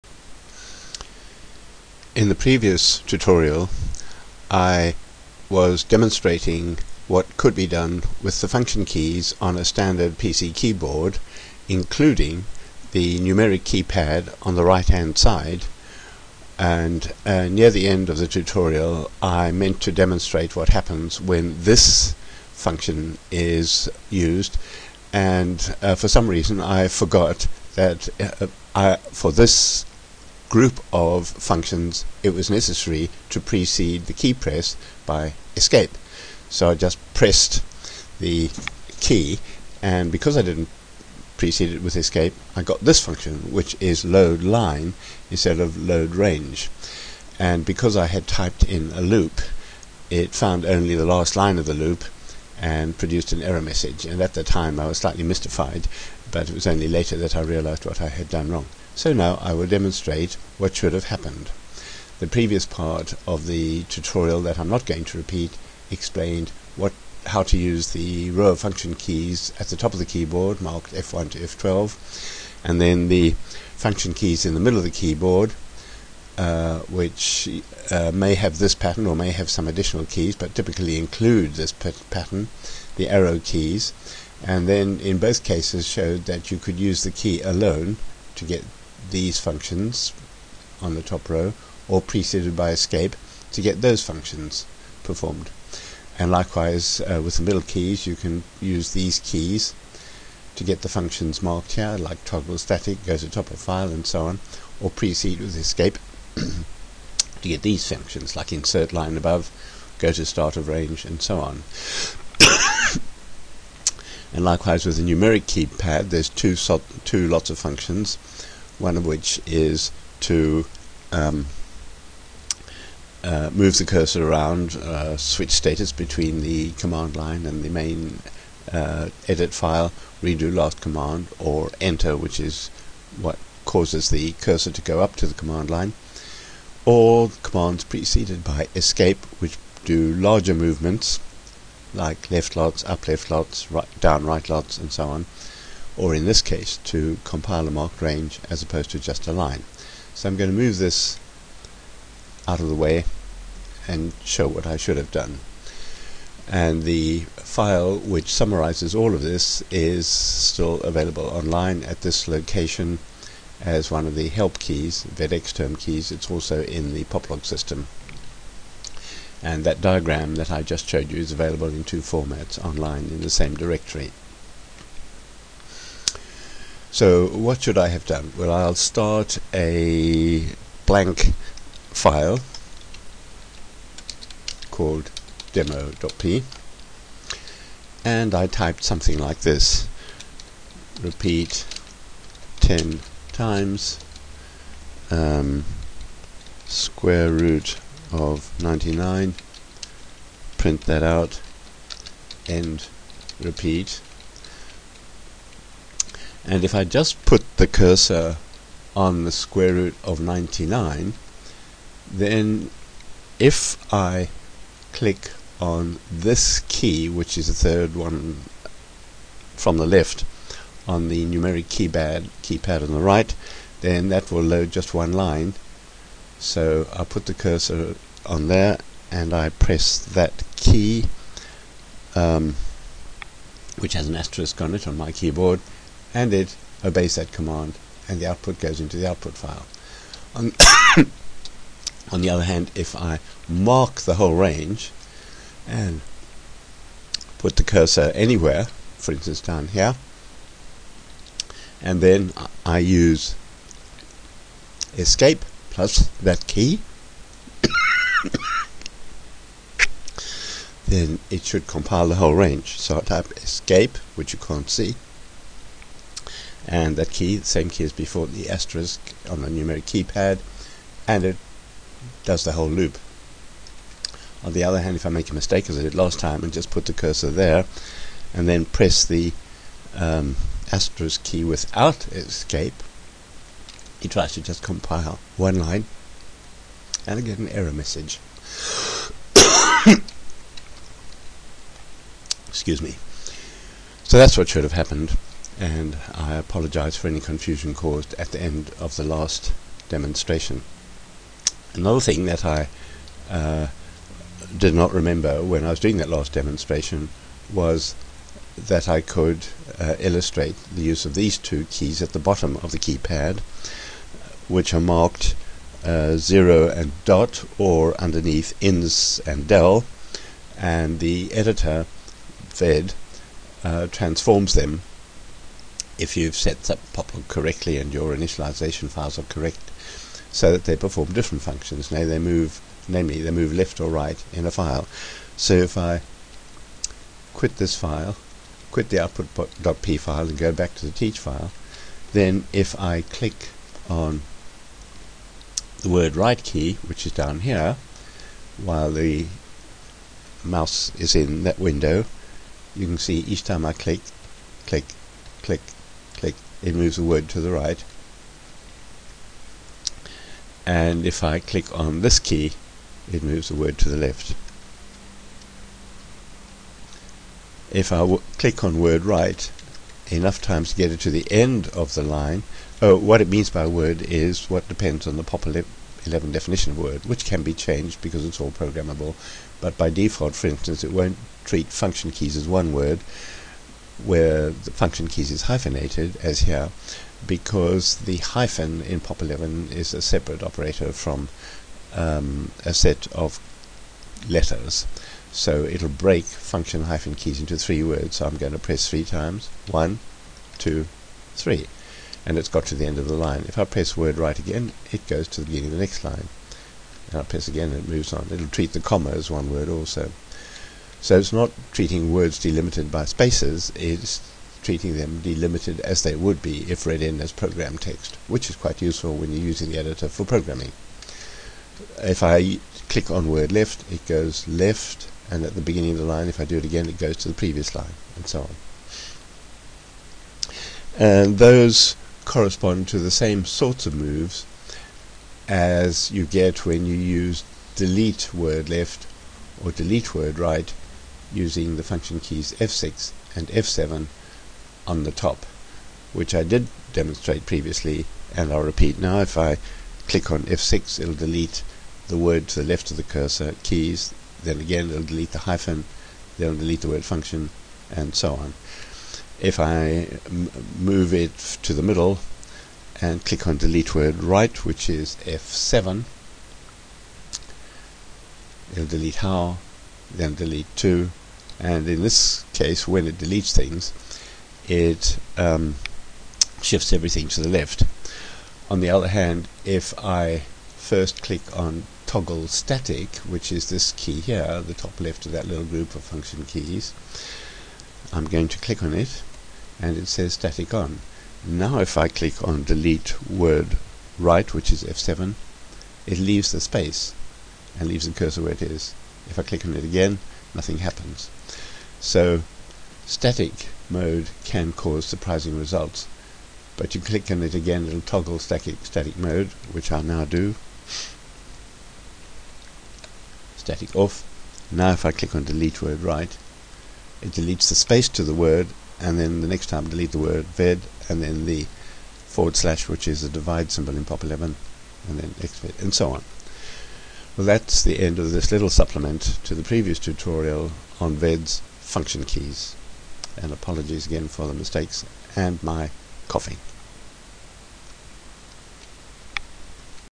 Using Numeric Keyboard to Compile a Range (load-marked-range -- correcting previous video)
In the previous tutorial, while demonstrating the use of numeric keypad keys in the poplog editor, I attempted to demonstrate the use of the '*' key on the keypad to compile a number of lines of Pop-11 code, which had previously been marked, using the Mark Start and Mark End function keys.

Unfortunately I forgot to press the ESC key before pressing the '*' key, so instead of compiling a range it attempted to compile only one line, and that line had 'endrepeat', which is not legal Pop11 unless preceded by 'repeat'. The result was an error message which, at the time, mystified me. See the last few minutes of this video

Later I realised what I should have done, and so this tutorial is a supplement to the previous one, partly repeating the previous one and also correcting the mistake at the end of the previous one.